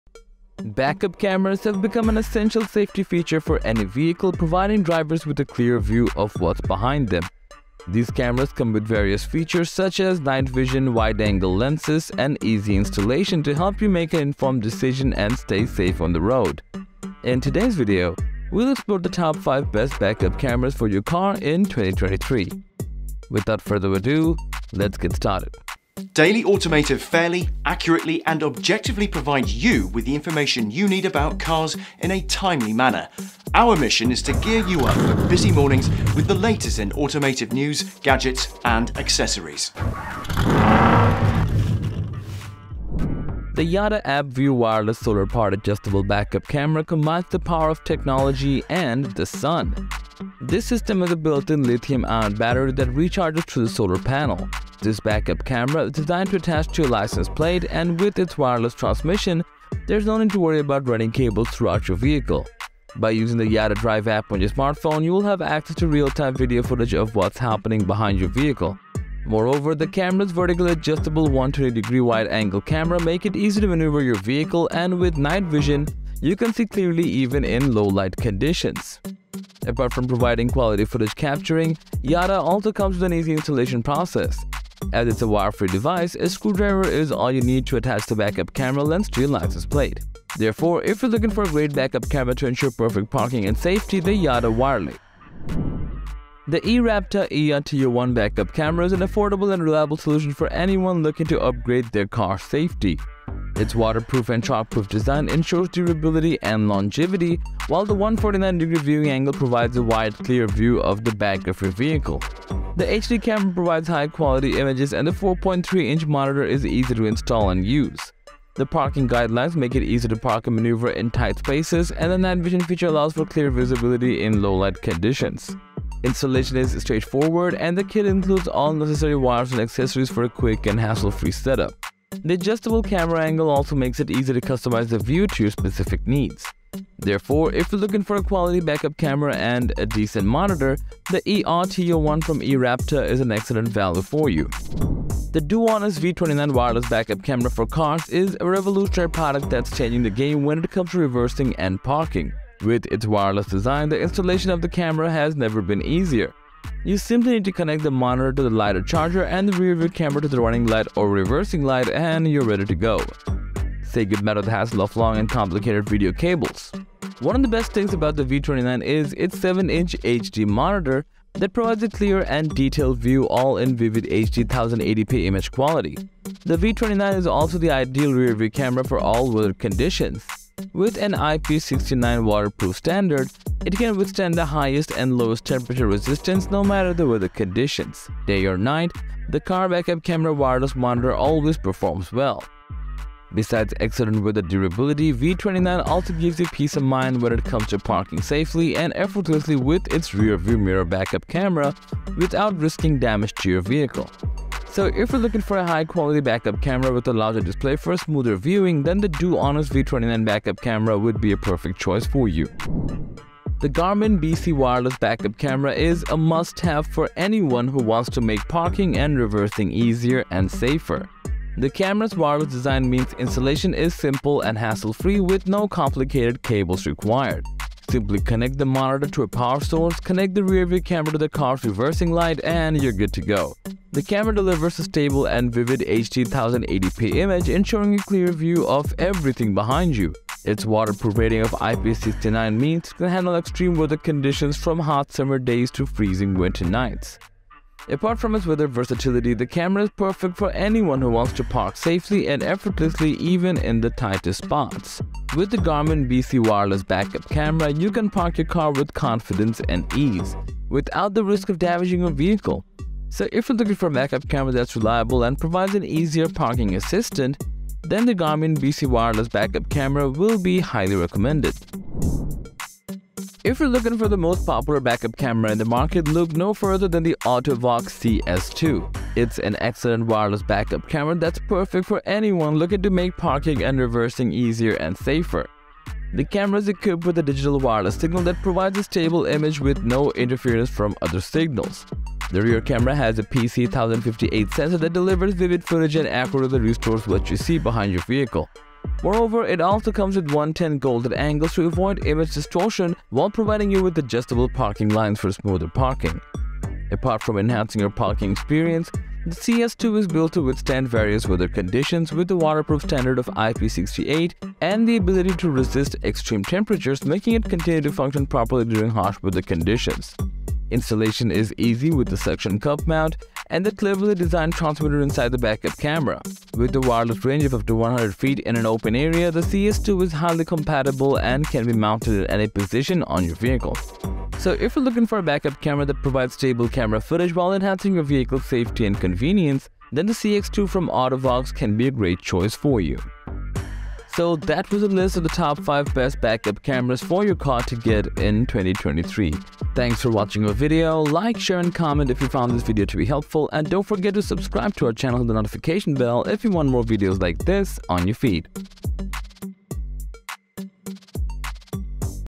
5 Amazing Backup Cameras in 2024
Backup cameras provide drivers with a clear view of what's behind them, which can prevent accidents and make parking easier. As backup cameras have become increasingly important for driver safety and convenience, that’s why in today’s video, let’s check the top 5 best backup cameras for 2024.

Top 5 Best Backup Cameras in 2024

5. YADA Backup Camera For Cars

4. eRapta ERT01 Backup Camera

3.  DoHonest Wireless Backup Camera

2. Garmin BC Wireless Backup Camera

1. AUTO-VOX CS-2 Wireless Backup Camera